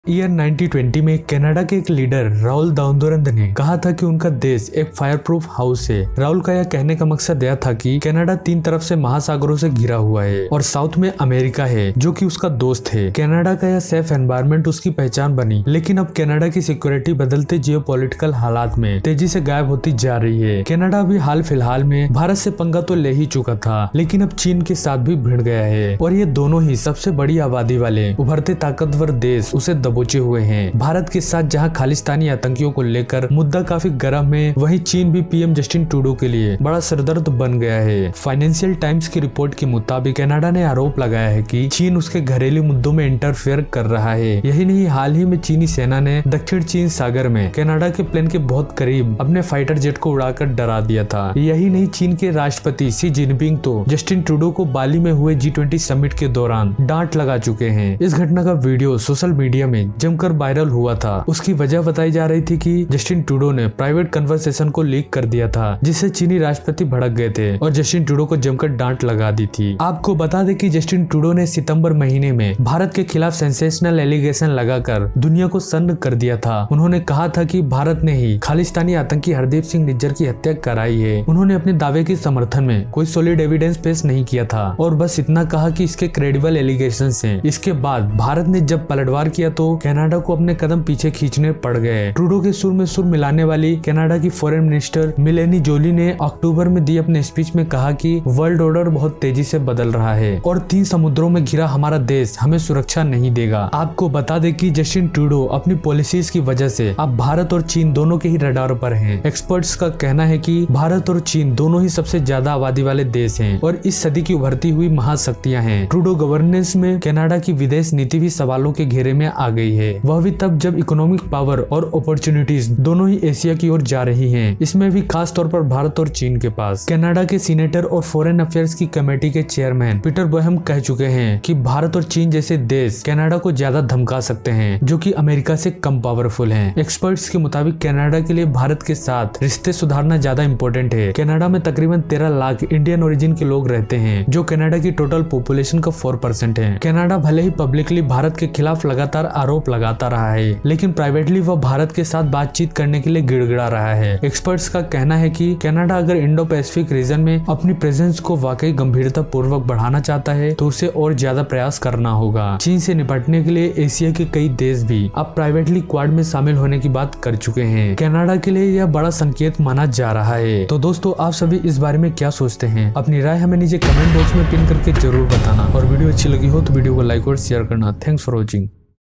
Canada भारत के सामने गिड़गिड़ाया , गलती सुधारना चाहता है Canada
Canada pleaded with India, wants to correct its mistake India Canada Relation

फाइनेंशियल टाइम्‍स की रिपोर्ट के मुताब‍िक कनाडा ने आरोप लगाया है कि चीन उसके घरेलू मुद्दों में Interfere कर रहा  है। यही नहीं हाल ही में चीनी सेना ने दक्षिण चीन सागर में कनाडा के plane के बहुत करीब अपने फाइटर जेट को उड़ाकर डरा दिया था।

According to the Financial Times report, Canada has alleged that China is interfering in its domestic issues. Not only this, recently the Chinese Army had given a scare by flying its fighter jet very close to a Canadian plane in the South China Sea.

This channel covers all national and international current affairs news.

 @Ago world